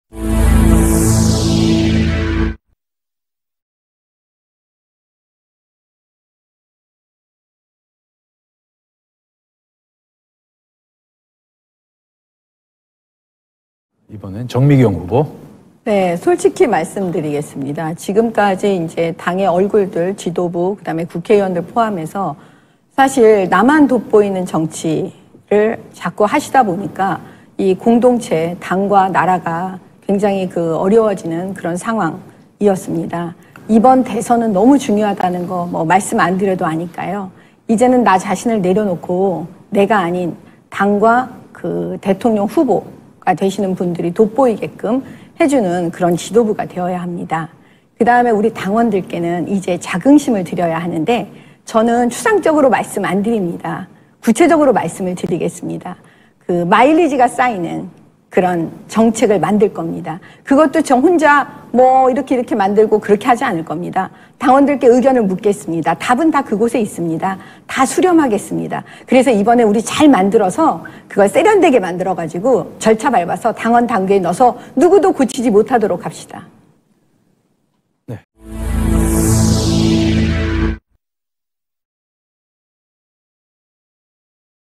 [데일리매거진/국민의힘 최고위원 후보 토론회] 정미경 후보 "당과 대통령 후보가 돋보이게 뒷받침하는 지도부가 되겠다"
국민의힘 전당대회가 2일 앞으로 다가온 가운데 최고위원 후보들의 경쟁도 치열해졌다.

주된 관심은 당대표 경선에 쏠려 있지만, 당 지도부의 일원인 최고위원 경선도 흥행몰이를 하고있다.

총 4명을 선출하는 최고위원 경선에 10명이 도전했다. 이중 김재원, 배현진, 정미경, 조수진, 조해진 후보가 당선권으로 분류되고 있다는 분석이다.

여성할당제가 적용되면 배현진·이영·조수진·정미경 후보 중 1명은 반드시 최고위원이 된다. 상위 4명 안에 여성 후보가 포함되지 않을 경우, 4위 득표자 대신 최다 득표한 여성 후보가 최고위원에 오르는 시스템이다.

그러나 정가에 따르면 배현진·조수진·정미경 의원이 유력 후보군으로 꼽히면서 여성할당제 적용 없이 자력으로 선출될 가능성이 높은 것으로 알려졌다.

이에 유력 후보군인 정미경 후보는 8일 오후 백범기념관에서 열린 국민의힘 최고위원 후보 토론회에서 "이번 대선은 매우 중요하다. 나 자신을 내려놓고, 당과 대통령 후보가 돋보이게 뒷받침하는 지도부가 되겠다"며 지지를 호소했다.